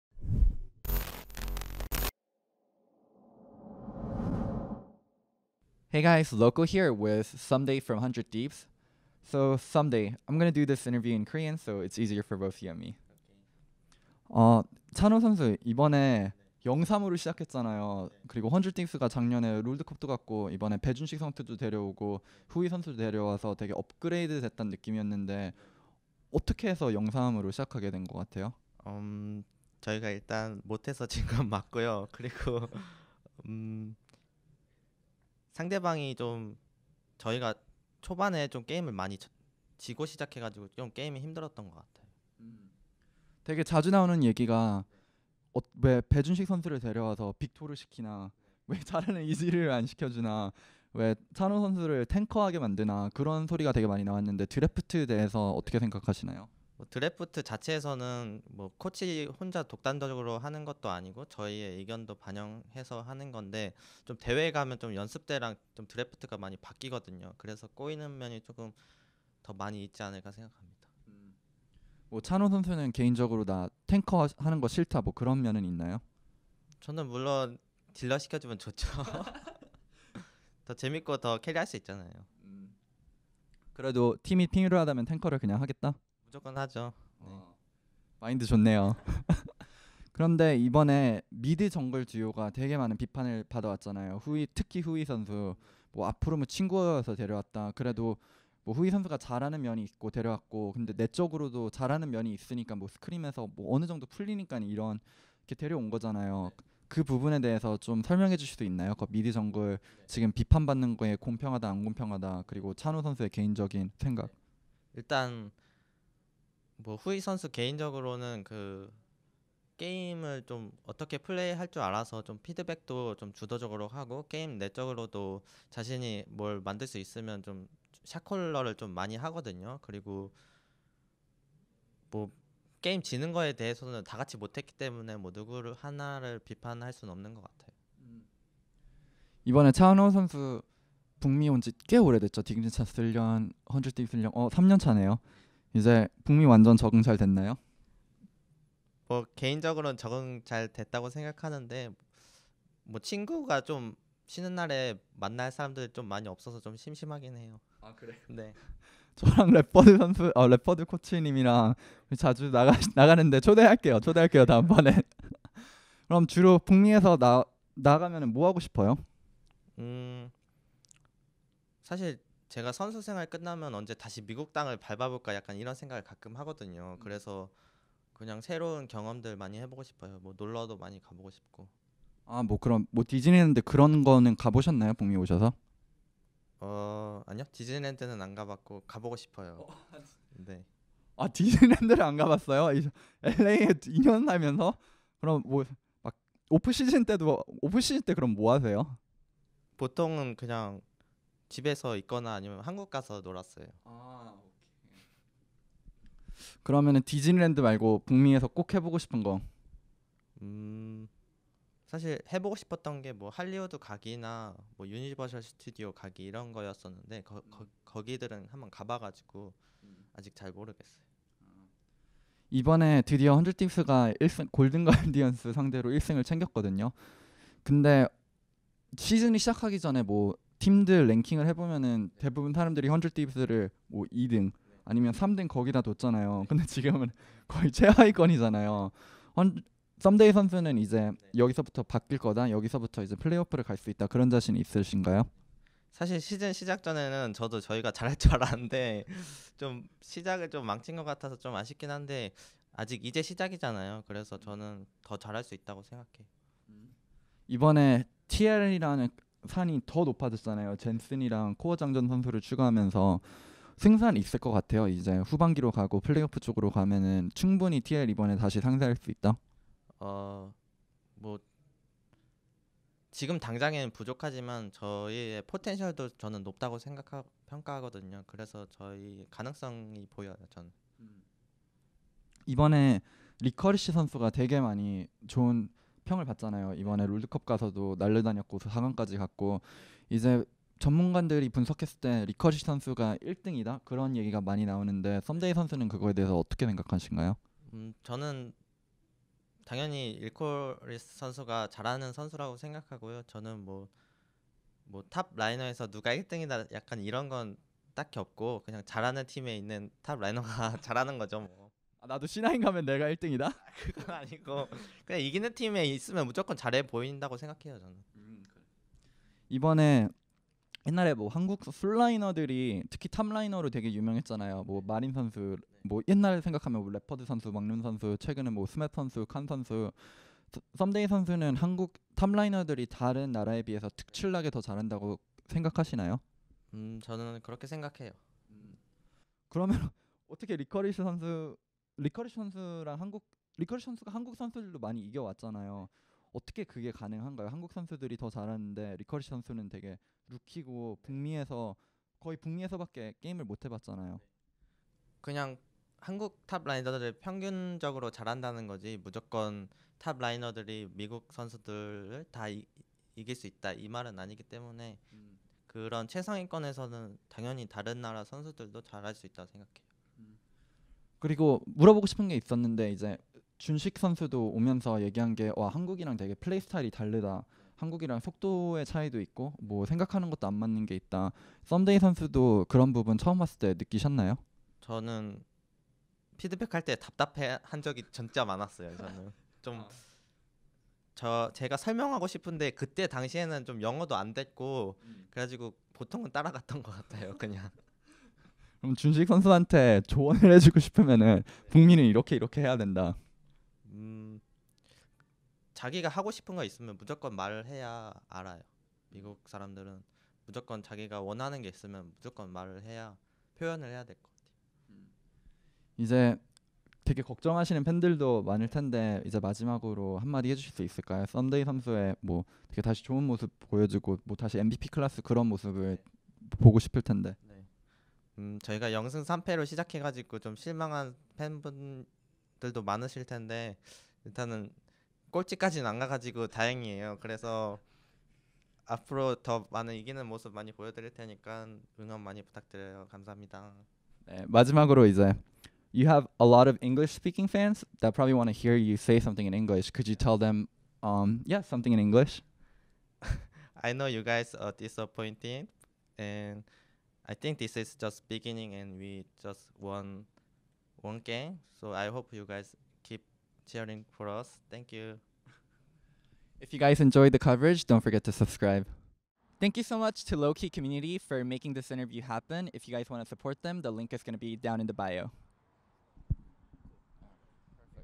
Ssumday's feelings on starting the split 0-3 and what he thinks of Licorice
⭐️⭐️⭐️⭐️⭐️ Open me ⭐️⭐️⭐️⭐️⭐️

100t was able to pick up a win this weekend and I was able to catch an interview with Ssumday